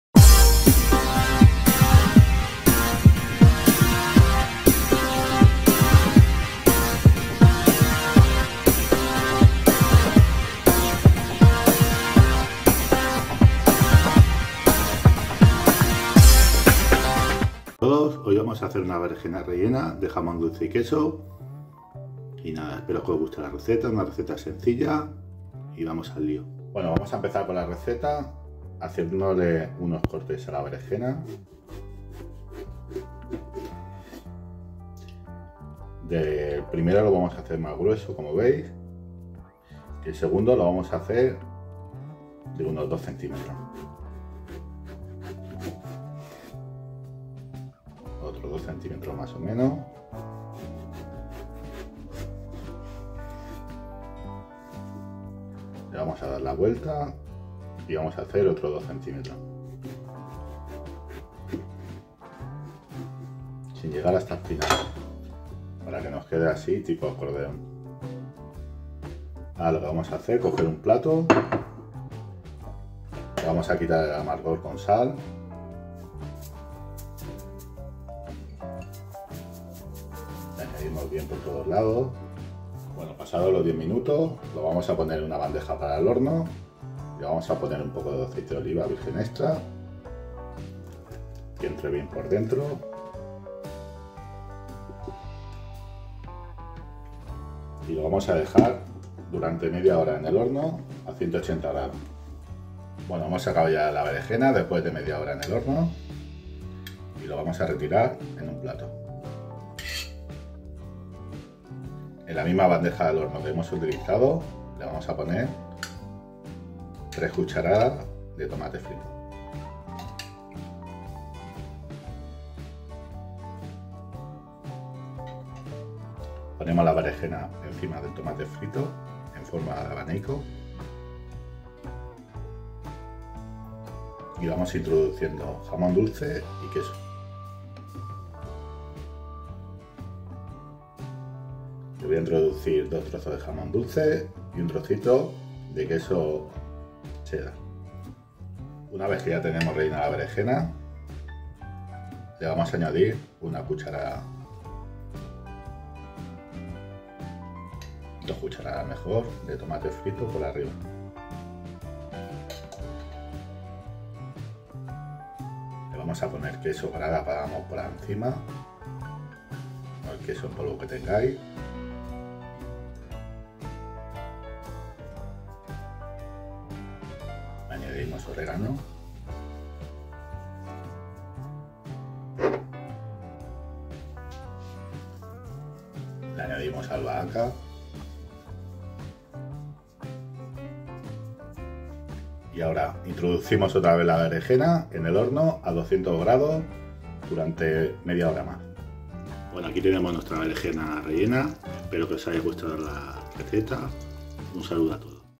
Berenjena gratinadas con jamón y queso
Ingredientes:
- 1 berenjena.
- 10 lonchas de jamón dulce.
- 5 trozos de queso.
- Queso en polvo.
- 5 cucharadas de tomate frito.
- Orégano y albahaca.
- Aceite de oliva.
- Sal.